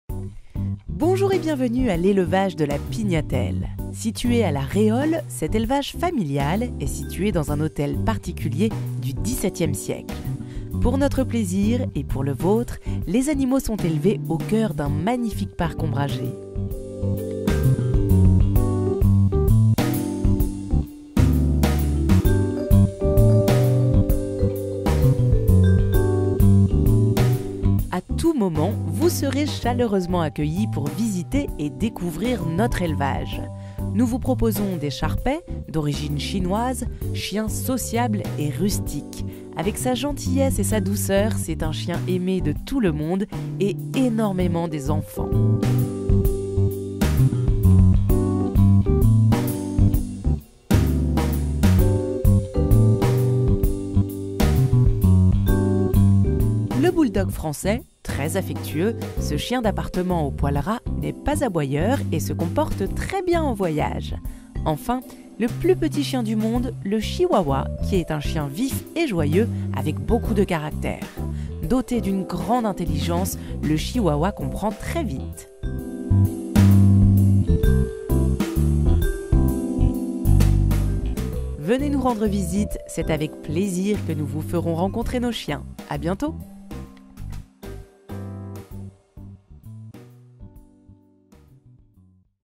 elevage la pignatelle
L'Elevage De La Pignatelle est situé à la Réole dans un hôtel particulier du 17e siècle. Ici vous y trouverez l'élevage de chiens de différentes races comme le bouledogue français ainsi que le Chihuahua.